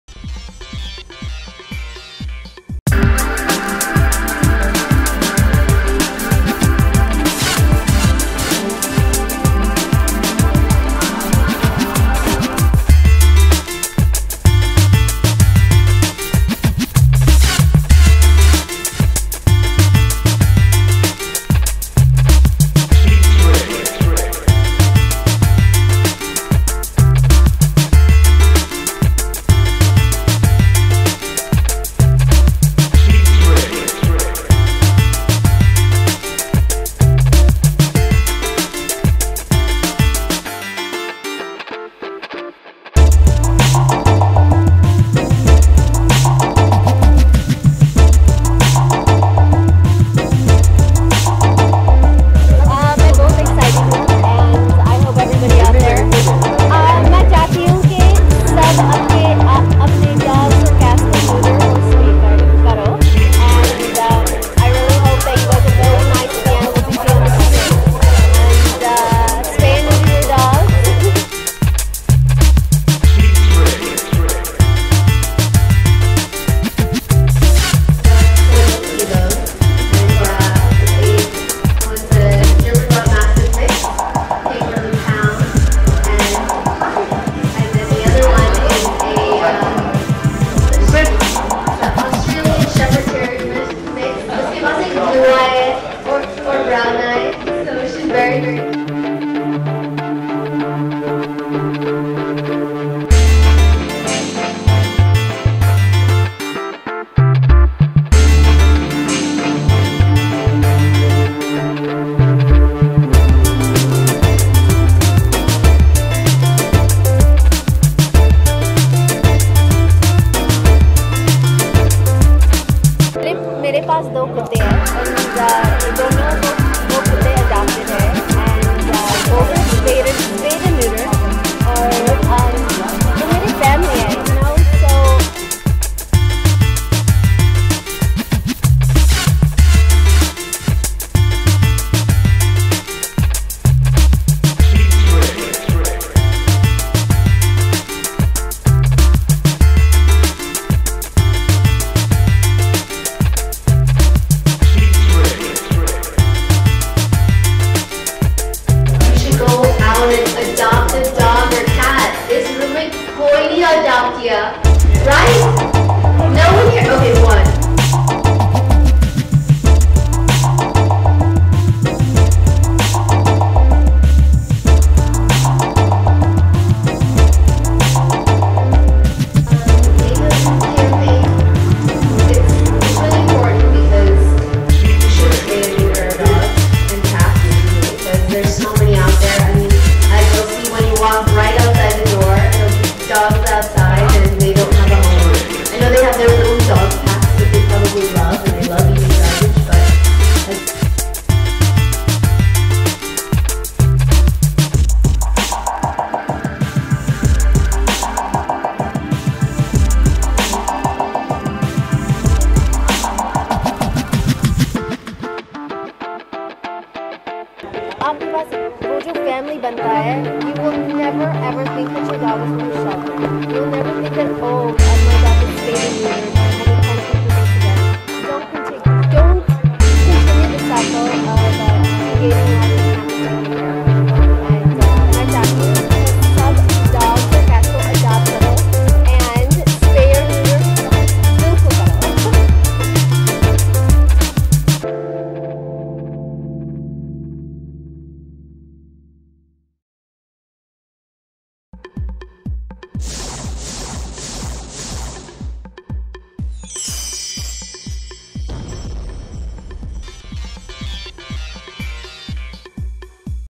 Sunny Leone's Take On Animal Protection at PETA
Sunny Leone was present to launch the new animal birth control ad of PETA in Mumbai. Sunny talked about his love for dogs, spreading awareness about the new PETA campaign and her upcoming film 'Ragini MMS 2'. Check it out !